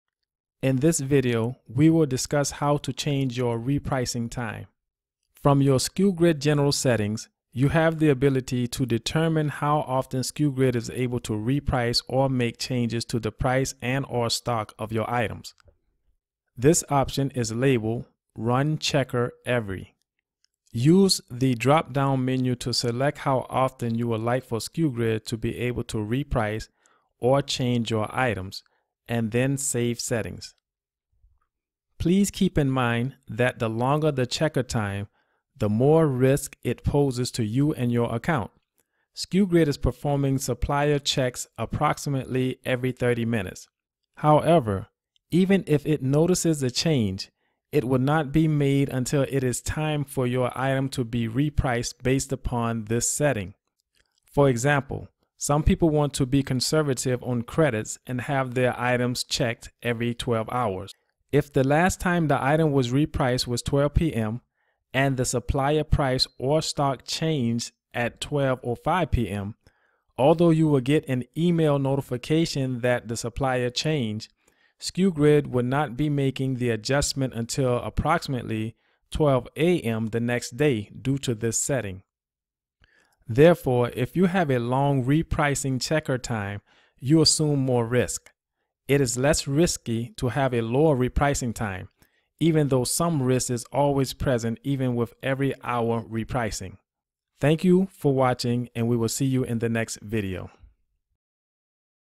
Sku Grid General Settings: How to Change Your Repricing Time
In this video, we will discuss how to change your repricing time.

From your Sku Grid General Settings, you have ability to determine how often Sku Grid is able to reprice or make changes to the price and/or stock of your items.

This option is labled Run Checker Every.

Use the drop down menu to select how often you would like for skugrid to be able to reprice or change your items and save settings.

Please keep in mind that the longer the checker time, the more risk that poses to you and your account. Sku Grid is performing supplier checks approximately every 30 minutes.  However, even if it notices a change, it would not be made until it is time for your item to be repriced based upon this setting.

For example, some people want to be conservative on credits and have their items checked every 12 hrs. If the last time the item was repriced was 12pm, and the supplier price or stock changed at 12:05pm, although you would get an email notification about the supplier change, skugrid would not be making the adjustment until approximately 12am the next day due to this setting.

Therefore, if you have a long repricing checker time, you assume more risk.  It is less risky to have a lower repricing time, although some risk is always present even with every 1 hr repricing.